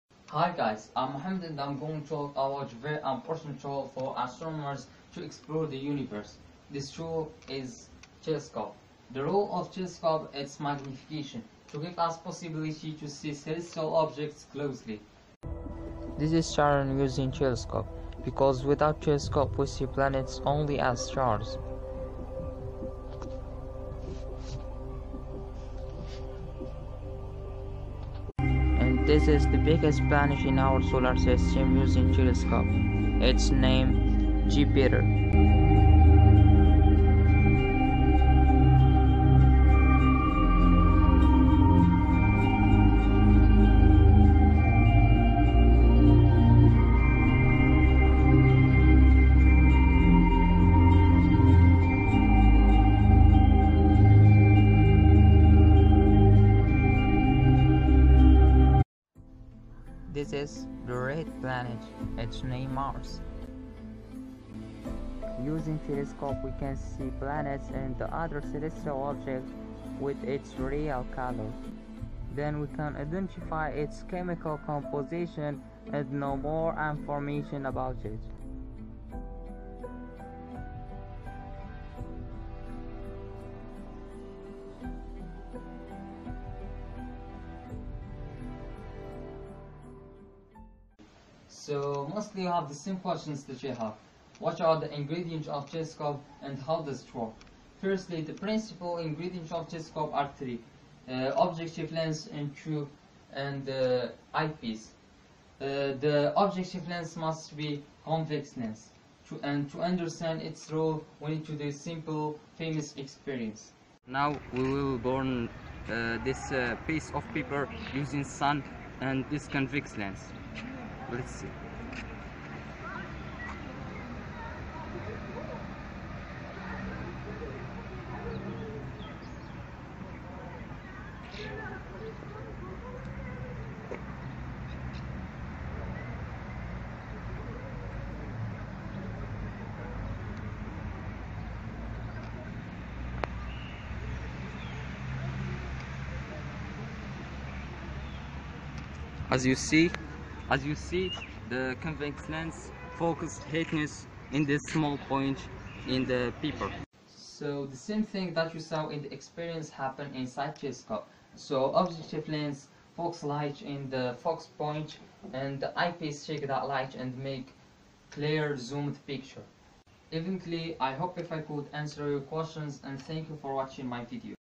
Racetospace How does refractor telescope work ? Mohamed Haimoud
Racetospace2020 I made This video for participate in " race2space " competition

Space Camp 2020